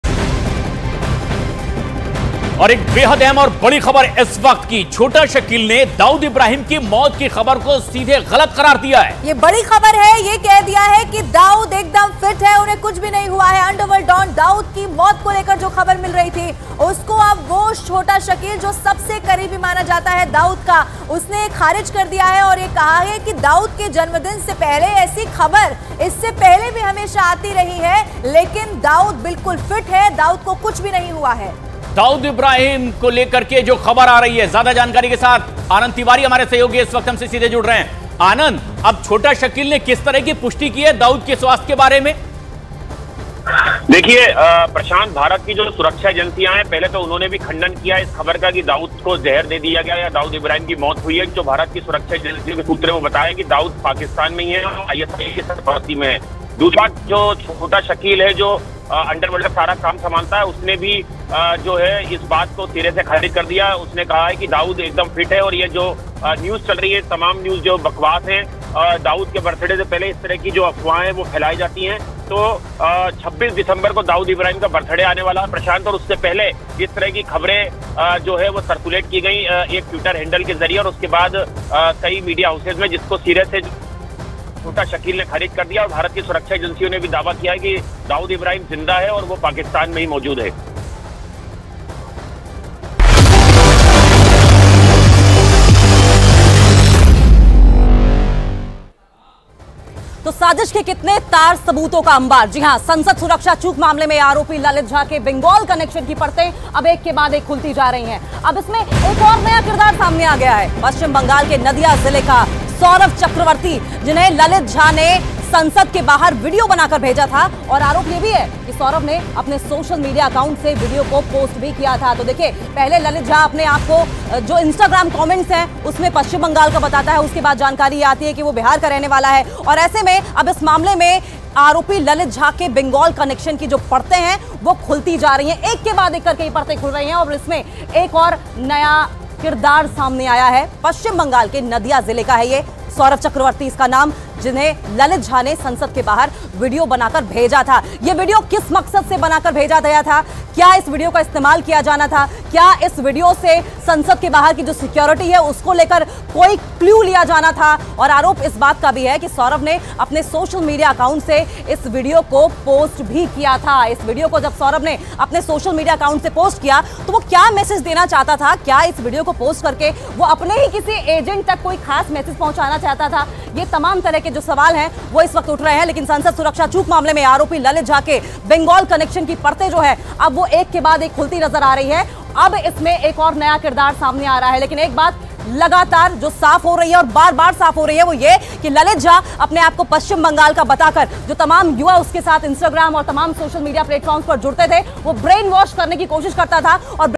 Breaking News : Dawood Ibrahim की मौत को छोटा शकील ने नकारा, कहा-दाऊद एकदम फिट हैं | News 18
There was a huge uproar in the Lok Sabha over the security lapse issue during the winter session of Parliament. After this the proceedings of the Lok Sabha were adjourned till 2 pm.

छोटा शकील ने दाऊद इब्राहिम की मौत की ख़बर को ग़लत बताया.. और कहा कि दाऊद एकदम फिट हैं..  उनके बर्थडे के पहले हर बात ऐसी न्यूज़ सामने आती है।